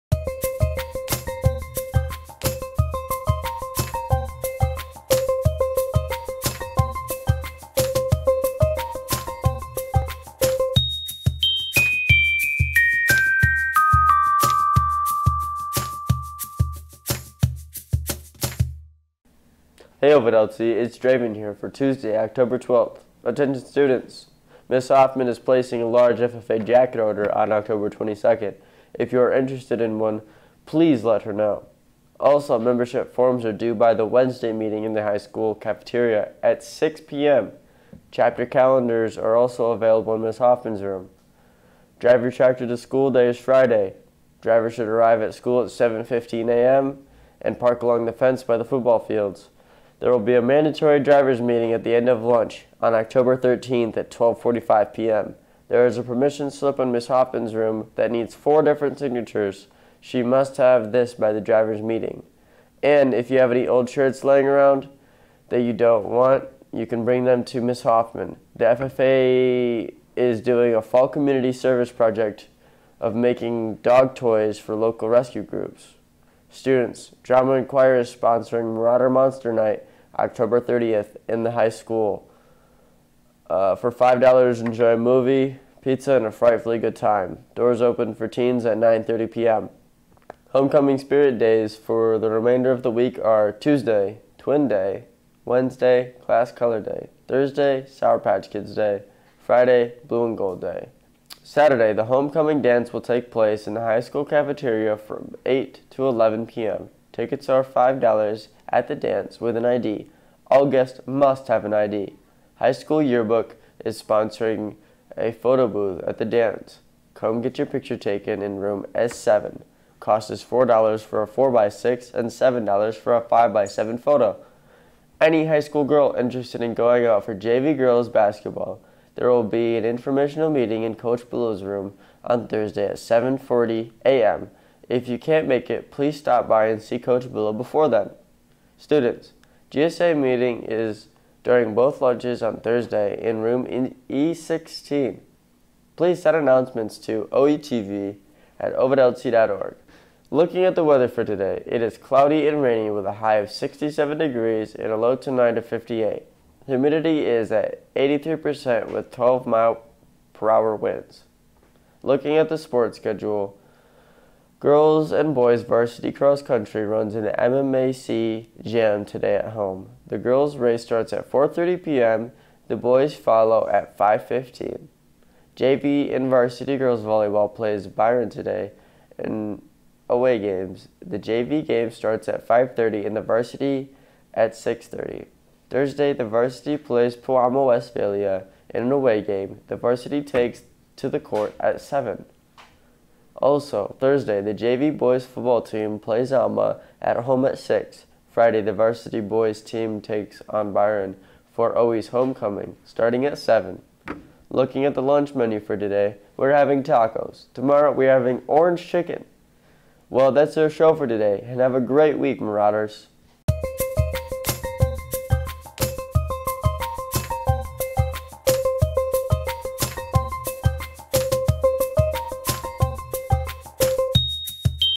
OETV Update for Oct 12, 2021
OETV Update for Oct. 12, 2021 with Dravin Silvercrow announcing (he has allergies). Ovid Elsie High School.